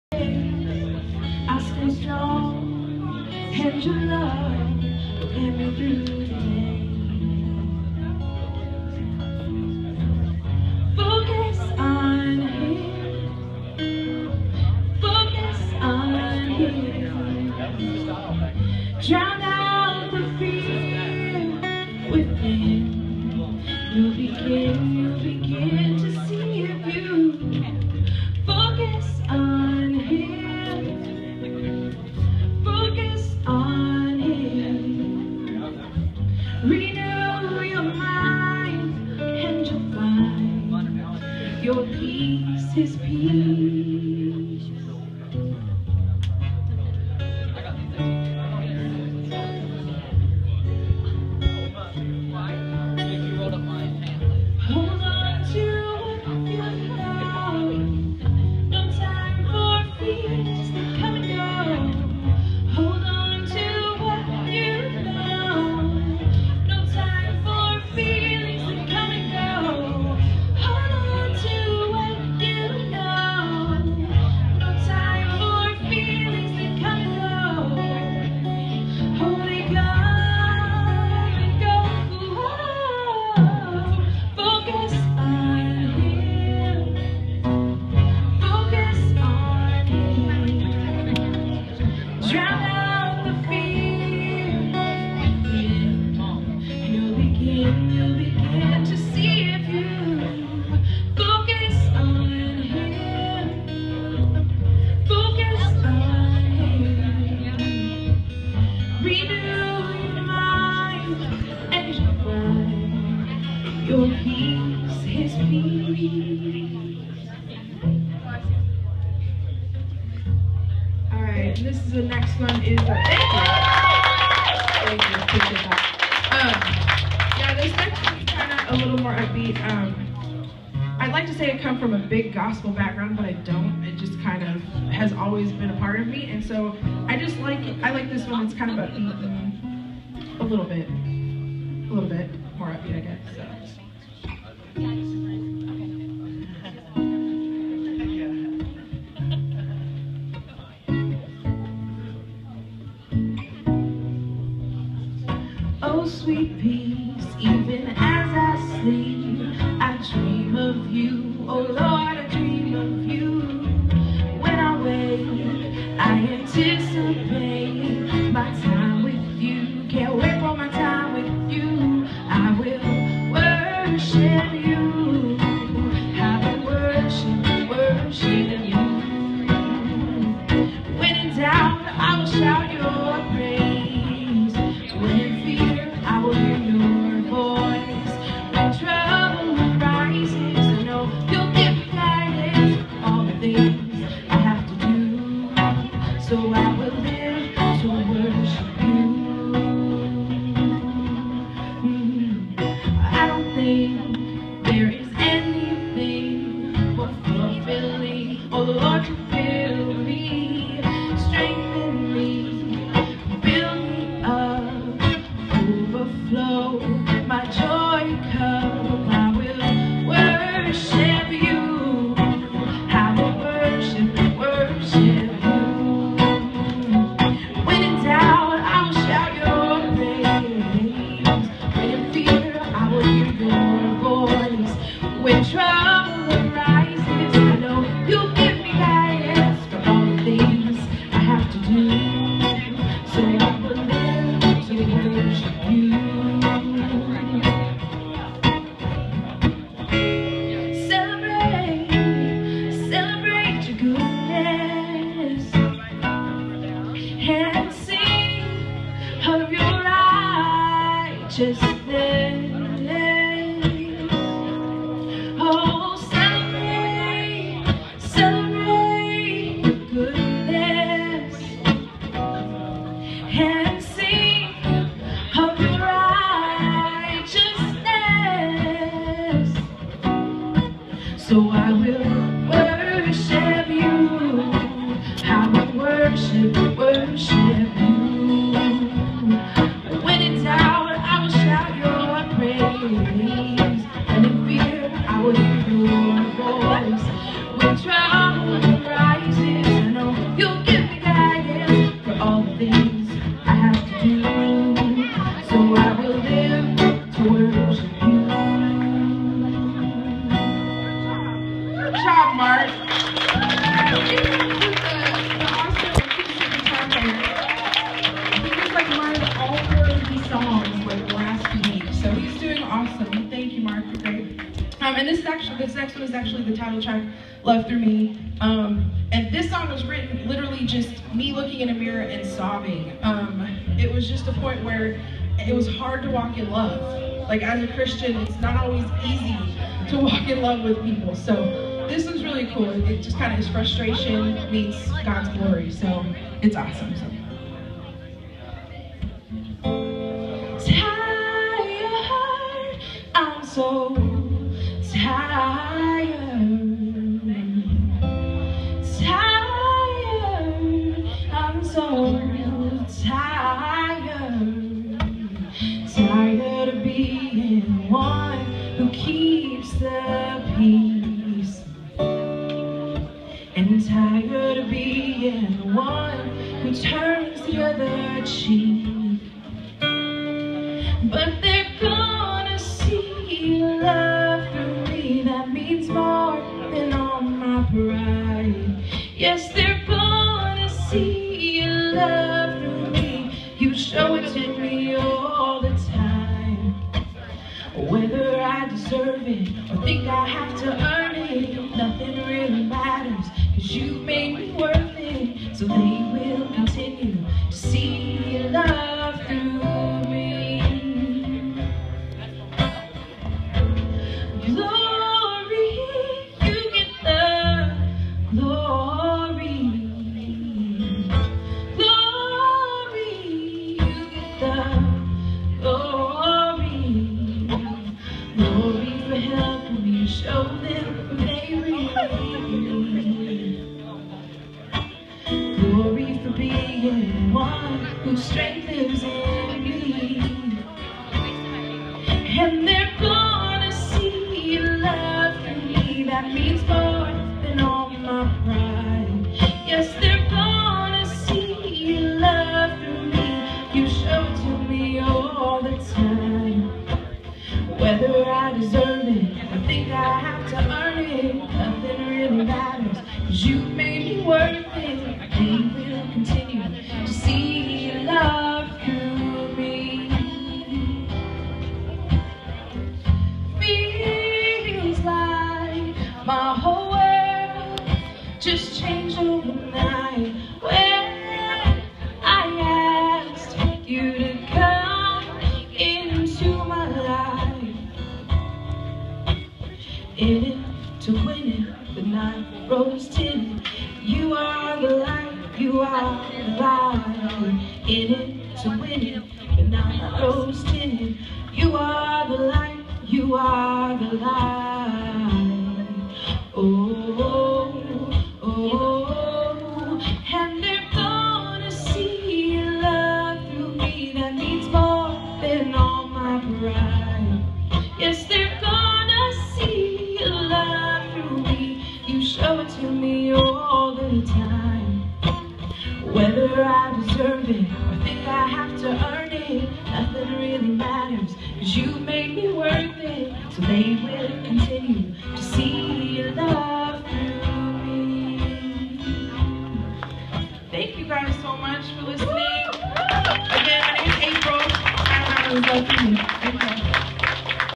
April Methven - Live at The Compound
Pre-release performance of songs from April's new album.